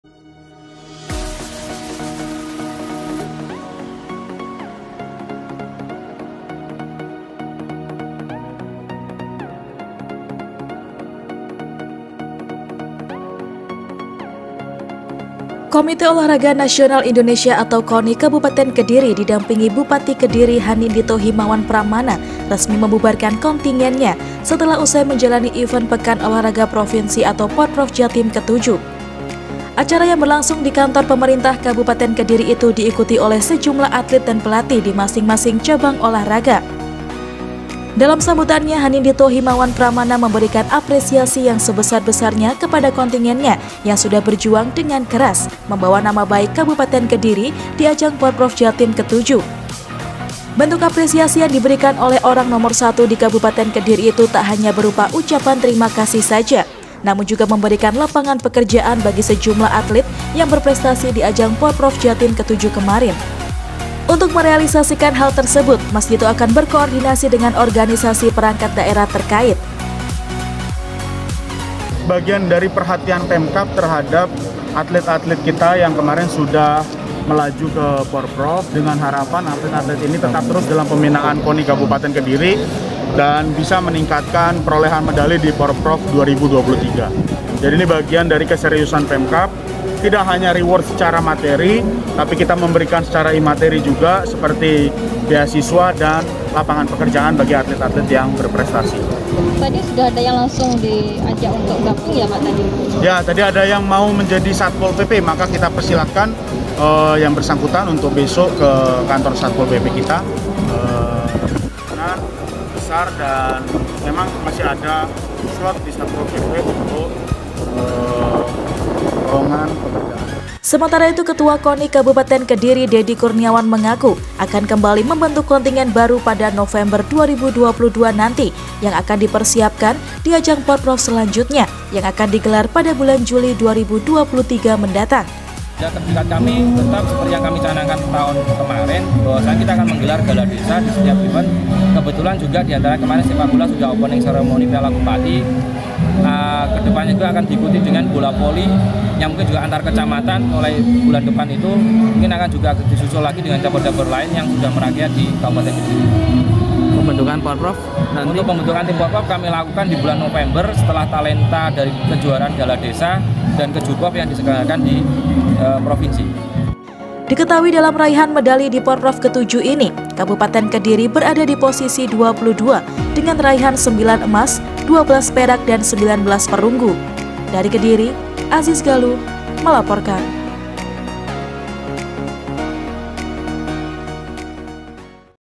Pembubaran Kontingen Porprov VII, Bupati Kediri Beri Lapangan Pekerjaan Kepada Atlet Berprestasi
Komite Olahraga Nasional Indonesia atau Koni Kabupaten Kediri didampingi Bupati Kediri, Hanindhito Himawan Pramana, resmi membubarkan kontingennya, setelah usai menjalani event Pekan Olahraga Provinsi atau Porprov Jatim ke VII.